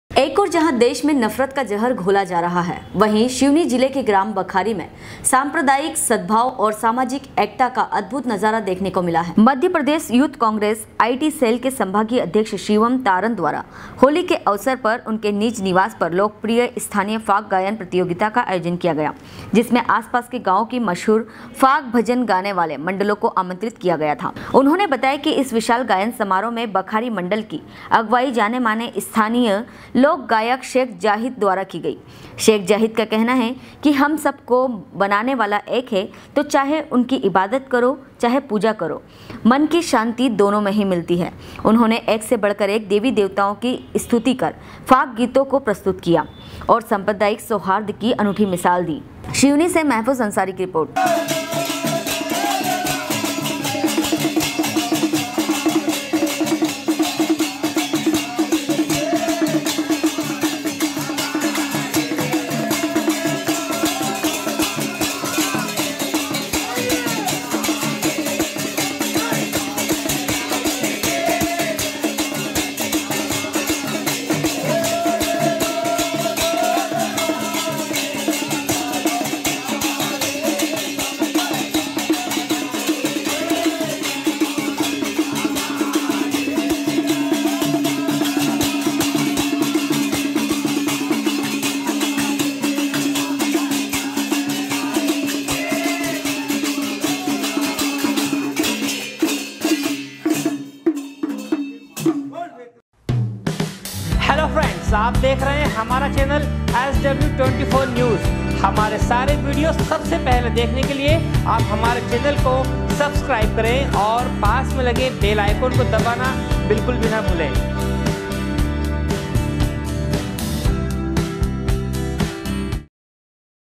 SW 24 News Madhyapradesh
एक ओर जहां देश में नफ़रत का जहर घोला जा रहा है वहीं सिवनी जिले के ग्राम बखारी में सांप्रदायिक सद्भाव और सामाजिक एकता का अद्भुत नज़ारा देखने को मिला है । सिवनी से महफूज अंसारी की रिर्पोट।